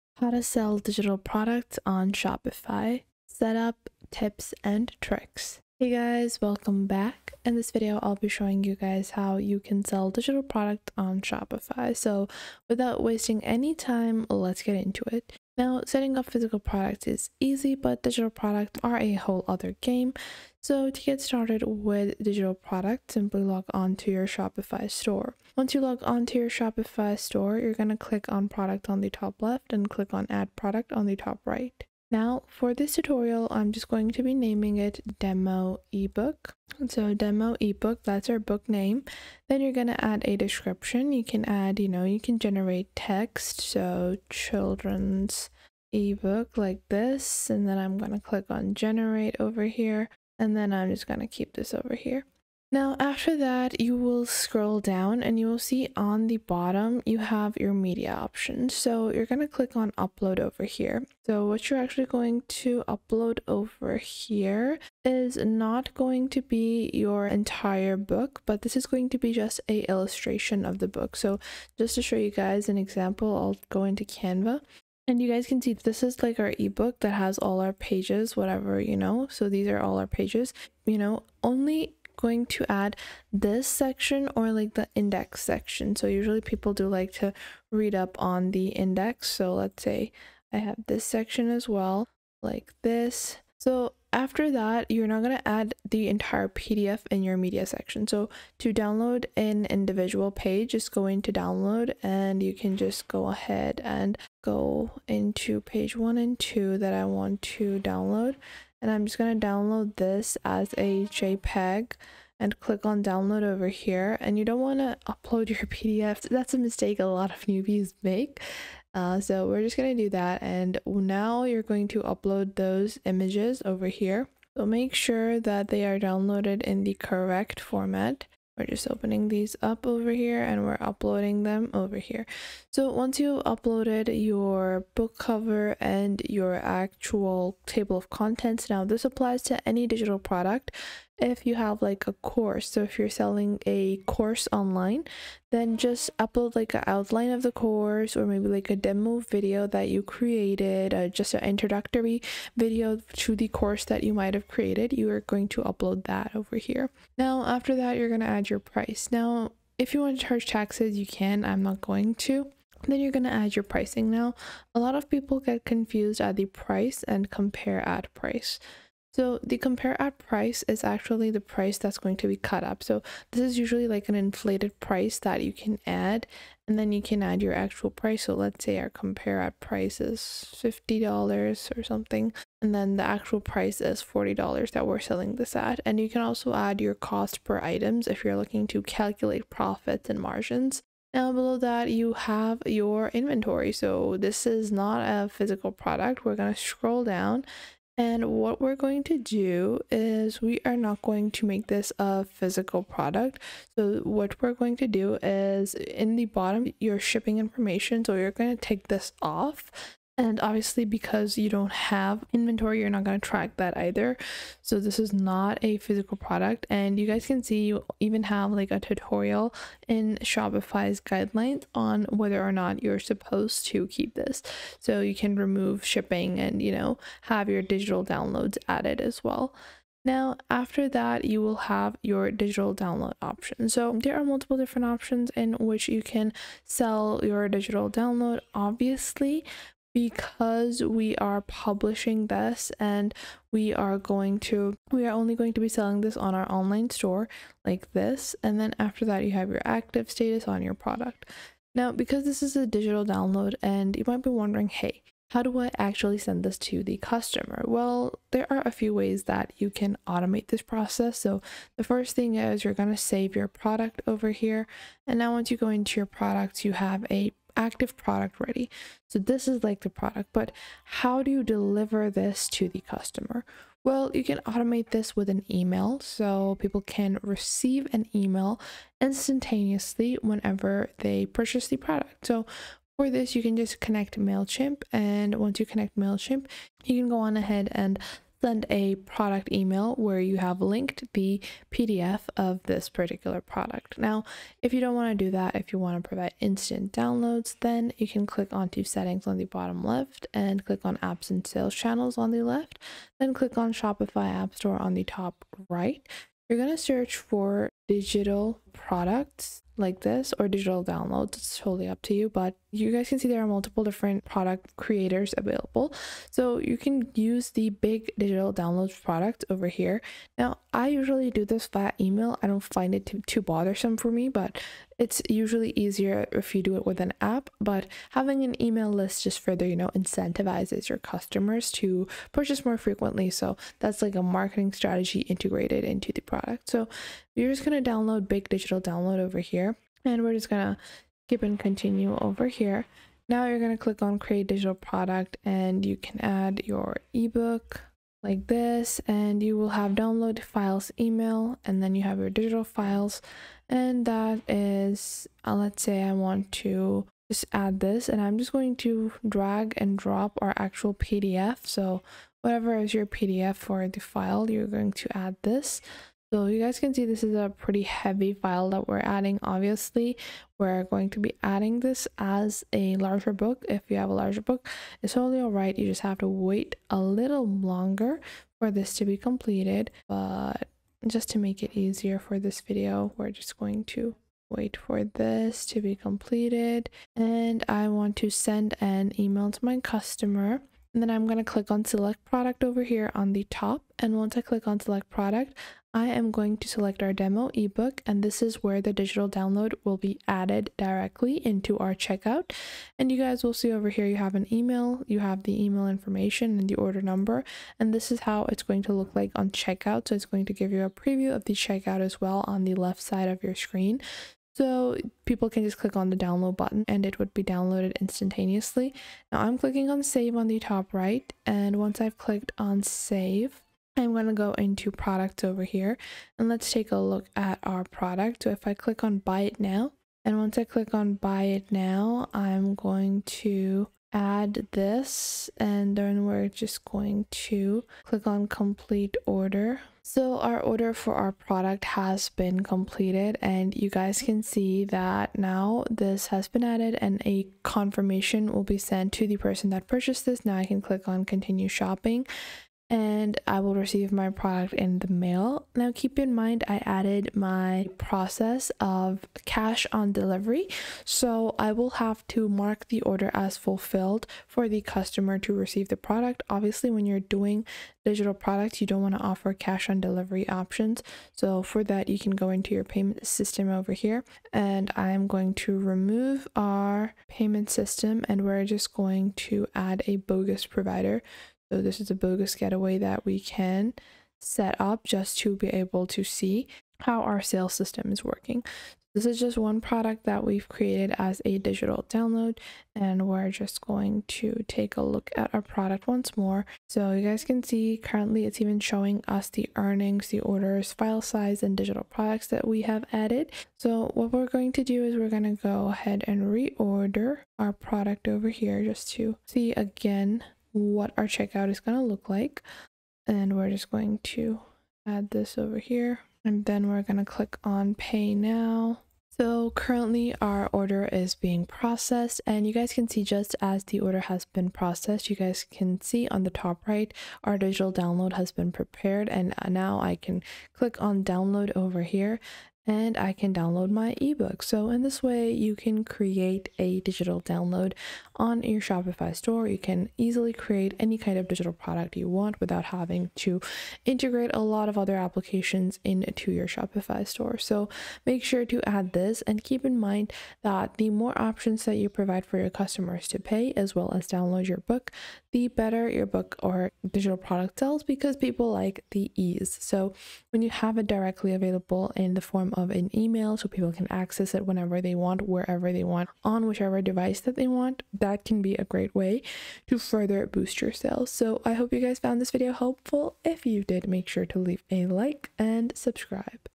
How To Sell Digital Products On Shopify 2026 (Setup + Tips & Tricks)
In this video, we show you how to sell digital products on Shopify step-by-step. This beginner-friendly tutorial covers everything you need to know to set up digital products, deliver files automatically, customize your storefront, and start selling successfully in 2026.